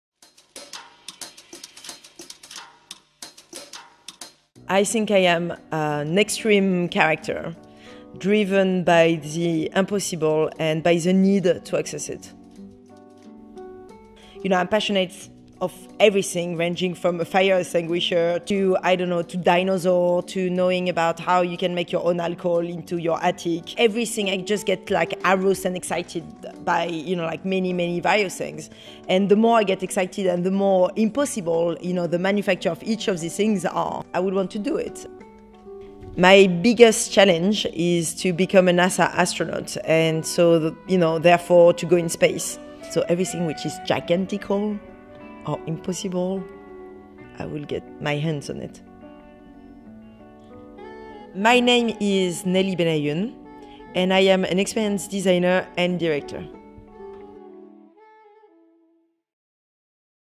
One Minute Wonder 37 Nelly Ben Hayoun
Nelly Ben Hayoun is an experience designer and director; she’s been called the “Willy Wonka of design and science”. Ben Hayoun is the brain behind the International Space Orchestra at NASA Ames Research Center . In addition to her role as director of ISO at NASA Ames Research Center, she is the Designer of Experiences at the SETI Institute and works at a variety of renowned research institutions. Her new project, Disaster Playground, investigates future outer space catastrophes and the design of procedures to manage them. In her spare time she is training to be an astronaut.